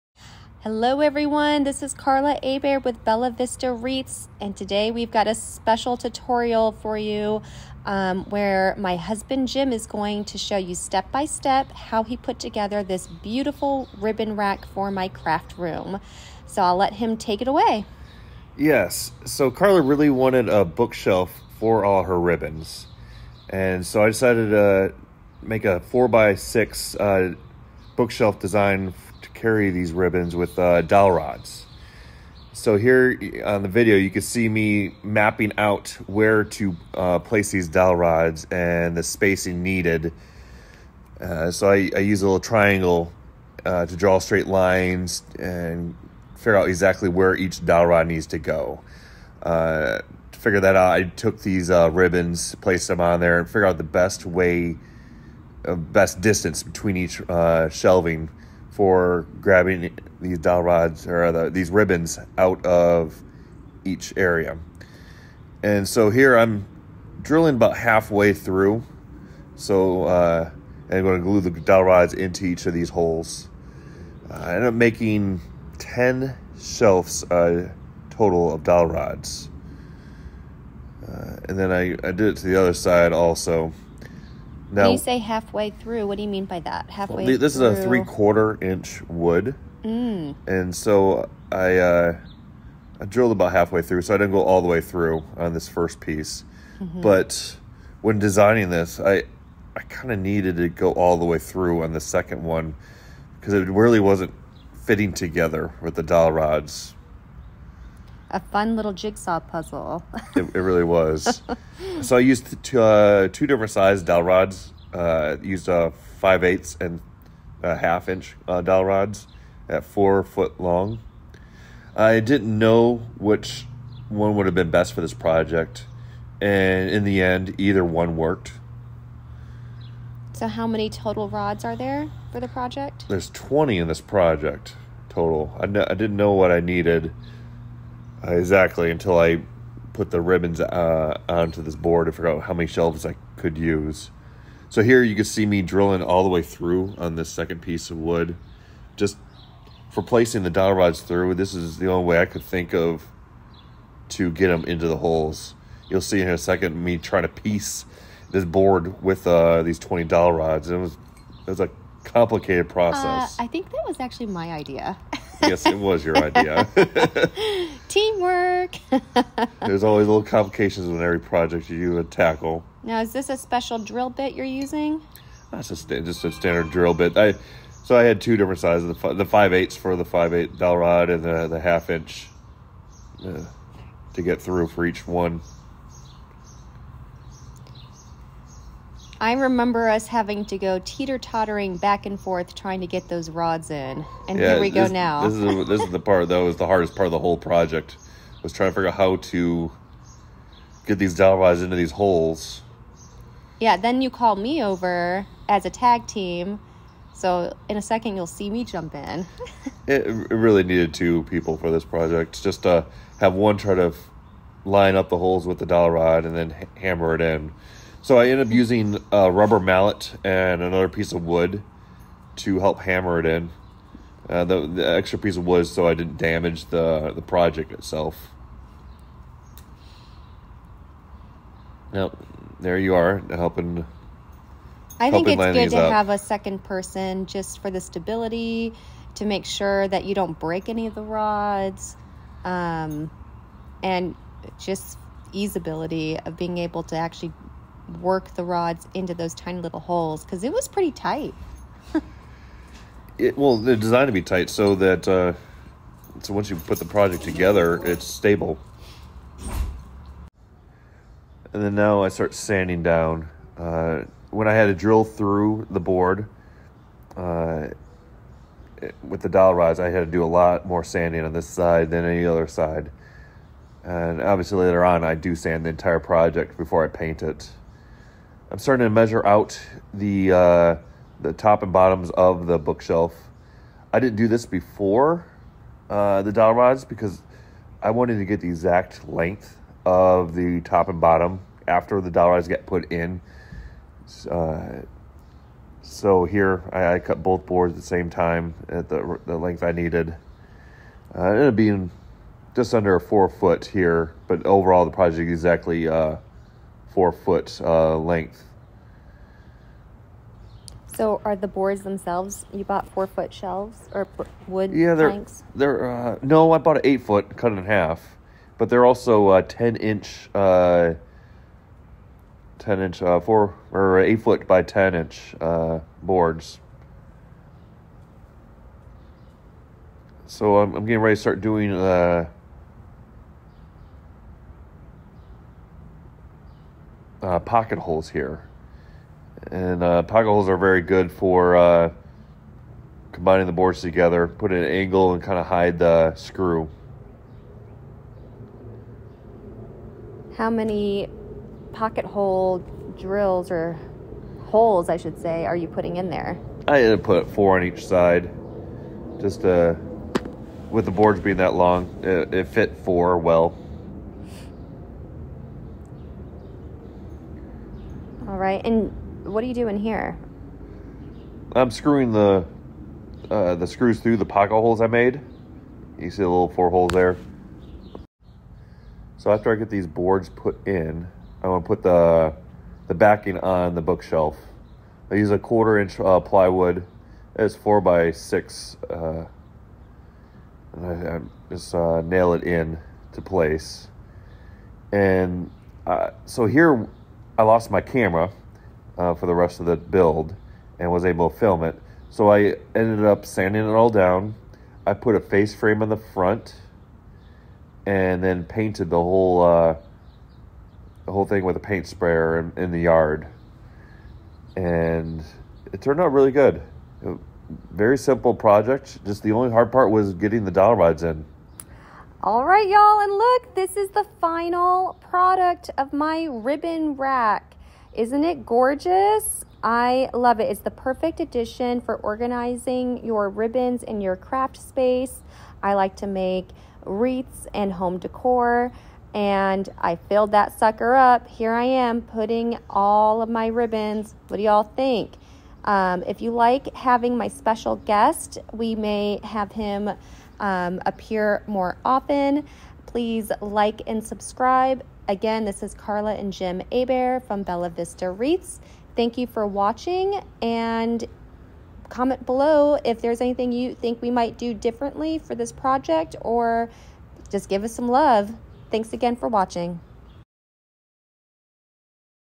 How to make a Ribbon Rack
In this video my husband, Jim Abair, will take you step by step on how he made my ribbon rack! This is the perfect addition to my crafting room and holds all my ribbons in one space! Looks like I’ll be needing another one soon though 😂

You can check out my wreath designs here at my Etsy Shop